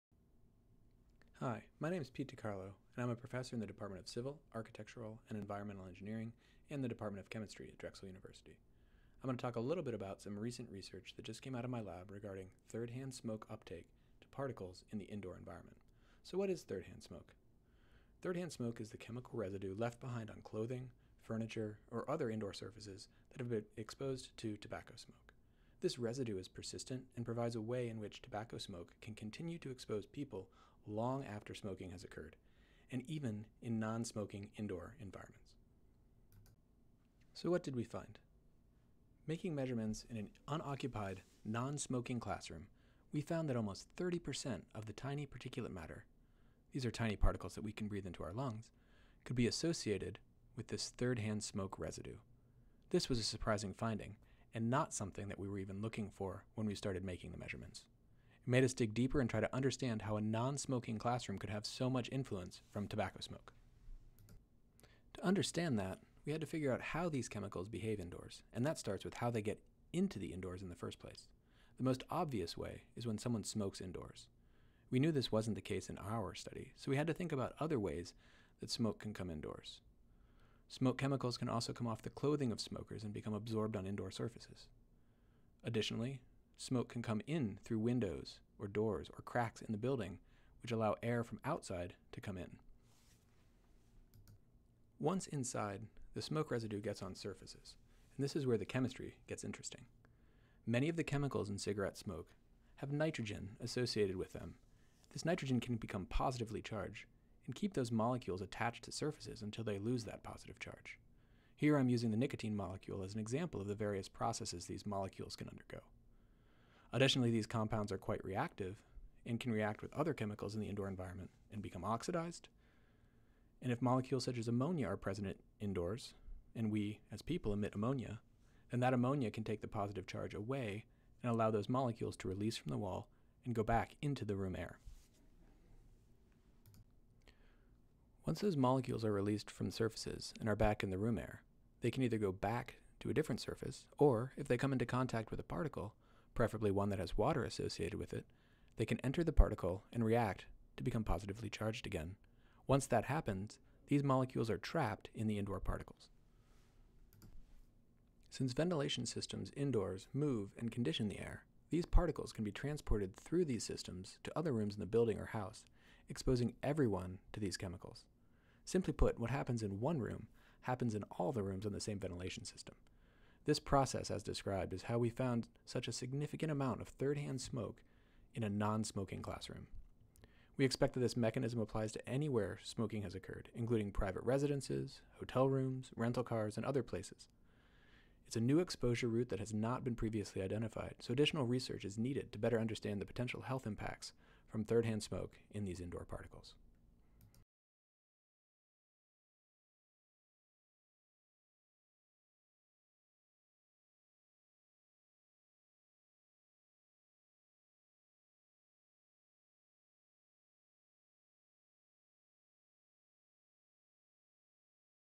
Third Hand Smoke Uptake to Particles Indoors
Third-hand smoke is the residue left behind by tobacco smoke.  This video describes how measurements made in a non-smoking classroom were able to identify the chemical signature of this residual smoke in indoor particles. These results combined with studies from other research groups were able to identify the mechanism by which this cigarette residue persists and makes its way into particles indoors where it can be transported around indoor spaces.  This means even non-smokers in non-smoking buildings can be exposed to these chemicals from tobacco smoke.  The original research article describing these results can be read for free at the following